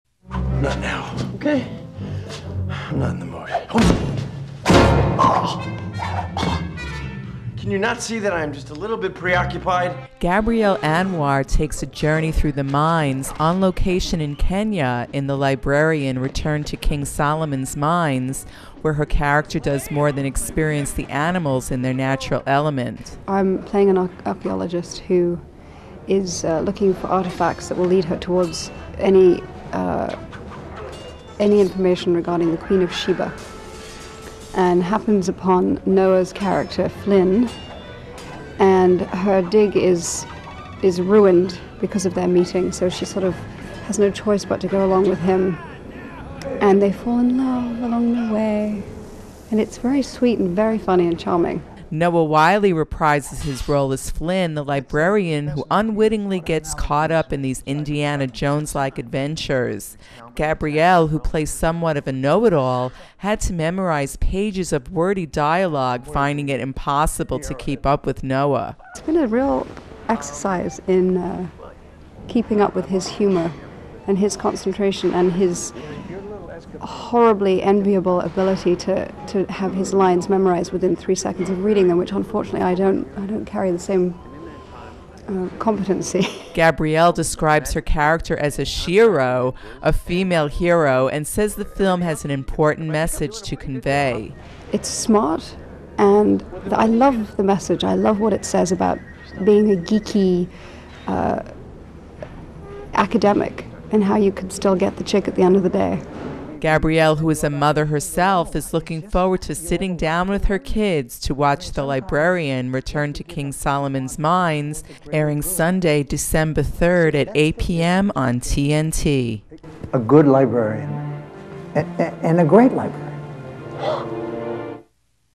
GABRIELLE ANWAR LIKES LIBRARIAN MESSAGE
Gabrielle Anwar stars in The Librarian: Return to King Solomon's Mines as an architect who meets the adventurous librarian. Keywords: Bob Newhart, Jane Curtin, Olivia Dukakis, Noah Wyle